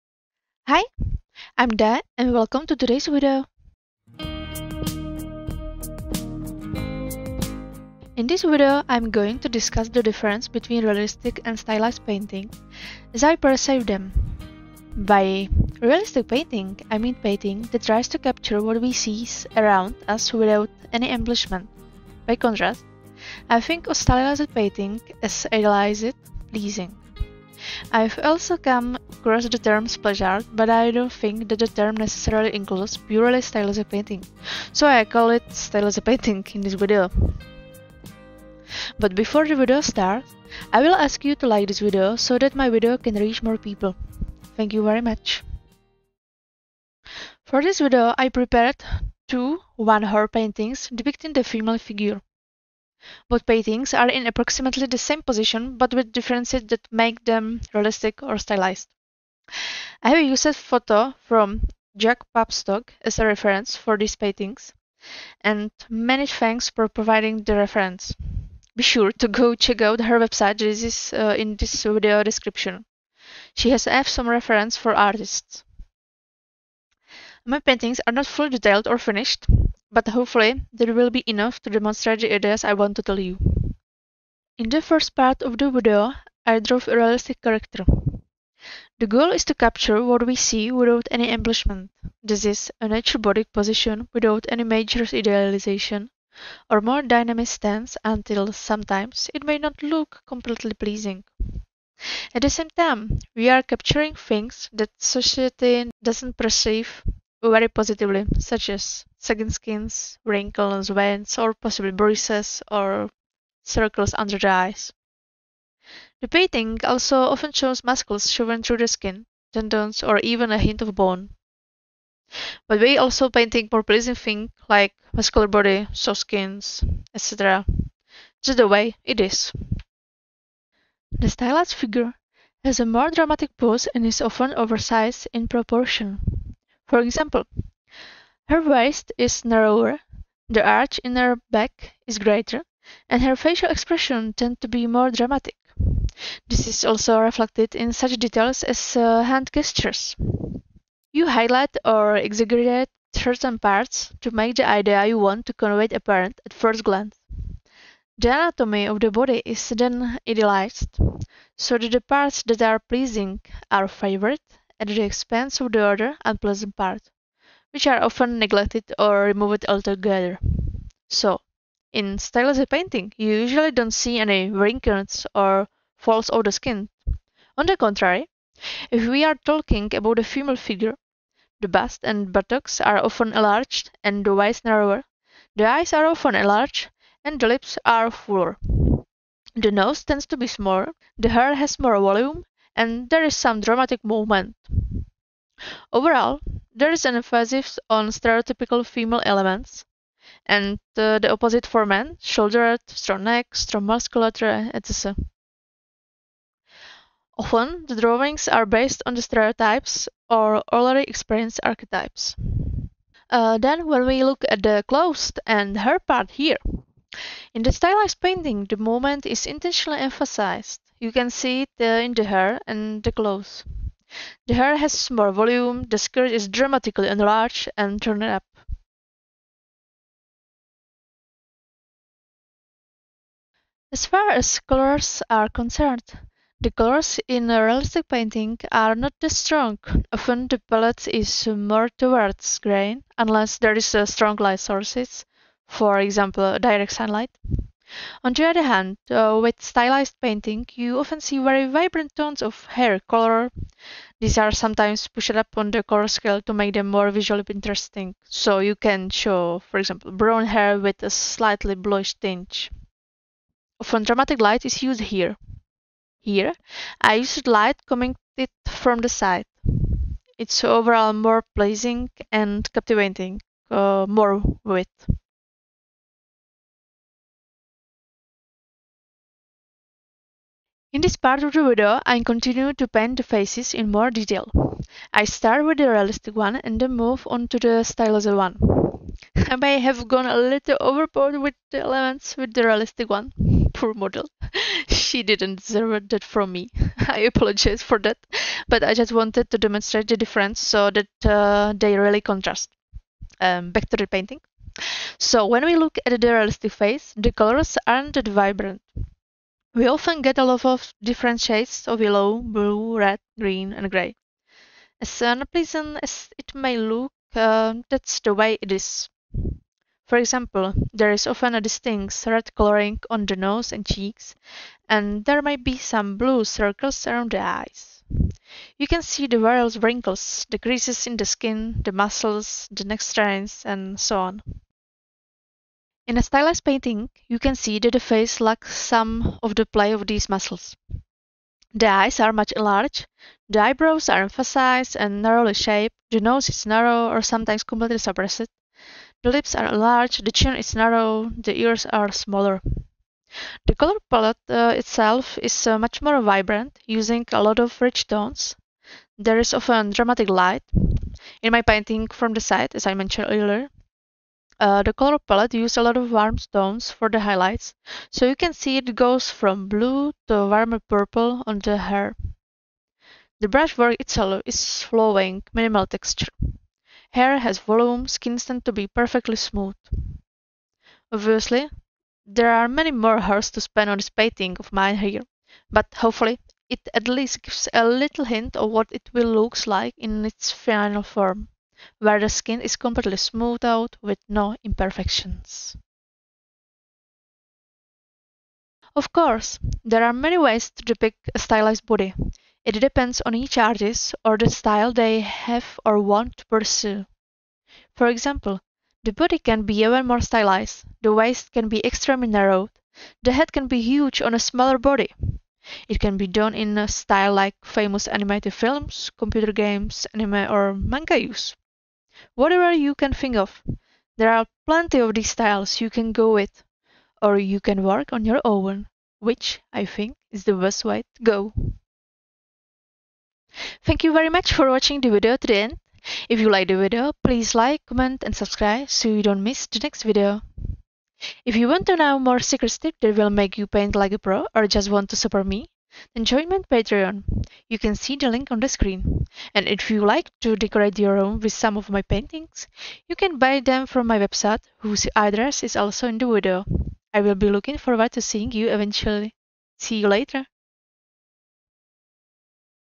Learn to understand the main differences between realistic and stylized painting - voiceover
Okay, my second youtube video. It's way out of my comfort zone, speaking in English, but I don't think I'll learn it any other way. :-)

Dae

*PSD files, steps, and normal speed painting process of my new paintings:*

CONTENT:
00:00 Introduction
00:46 Realistic body shape
02:10 Stylized body shape
03:33 Clothes
03:53 Colors
04:40 Faces - realistic
05:35 Faces - stylized
06:37 End